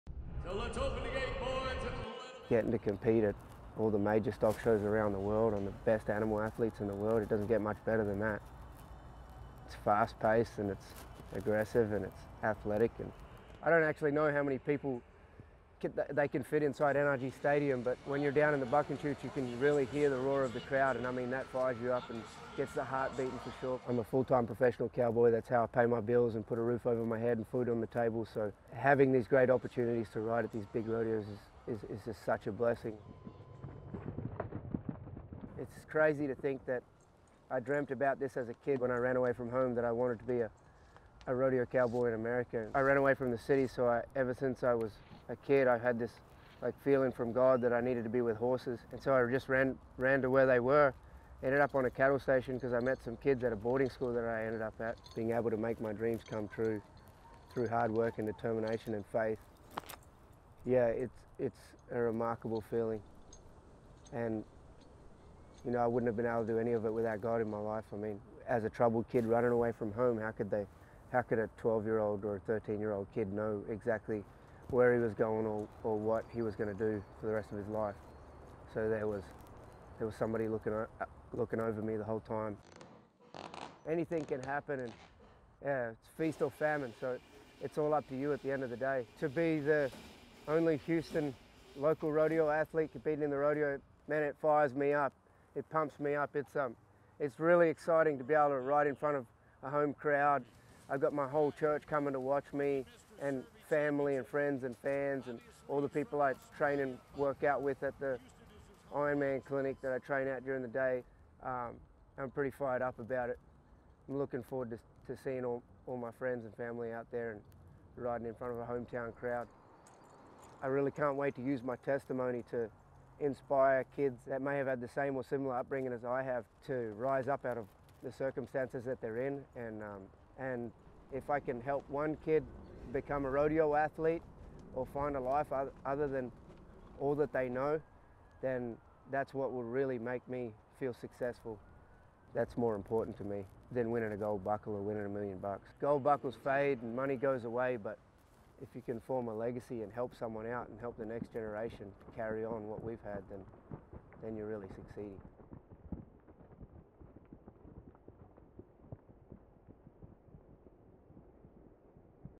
Rodeo Life: Anthony Thomas
Aussie Anthony Thomas lives out his dream of being an American Rodeo Cowboy at the Houston Live Stock Show and Rodeo